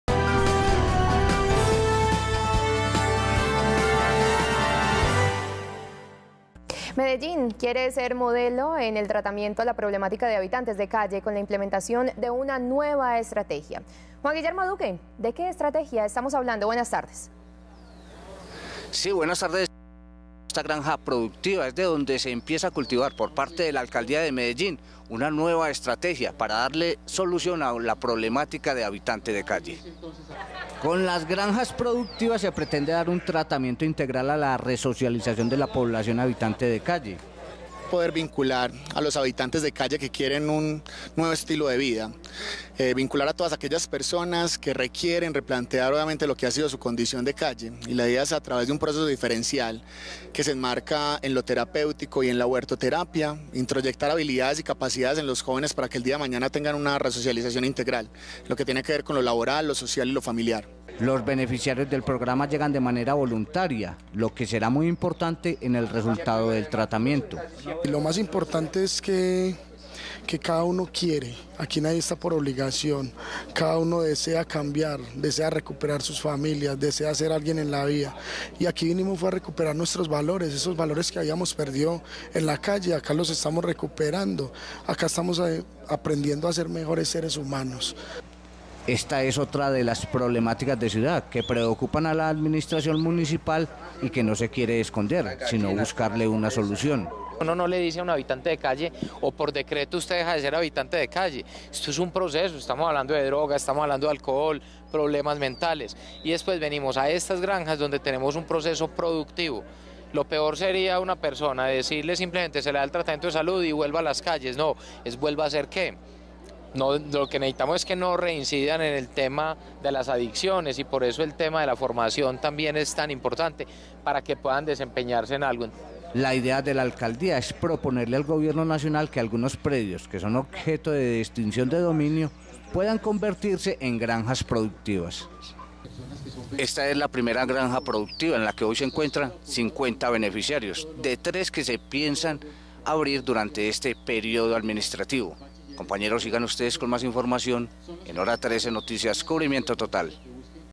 Con granjas productivas se pretende dar tratamiento integral a población habitante de calle
Con las granjas productivas se pretende dar un tratamiento integral a la resocialización de la población habitante de calle.
Los beneficiarios del programa llegan de manera voluntaria, lo que será muy importante en el resultado del tratamiento.
Esta es otra de las problemáticas de ciudad que preocupan a la Administración Municipal y que no se quiere esconder, sino buscarle solución.
La idea de la Alcaldía es proponerle al gobierno nacional que algunos predios que son objeto de extinción de dominio puedan convertirse en granjas productivas.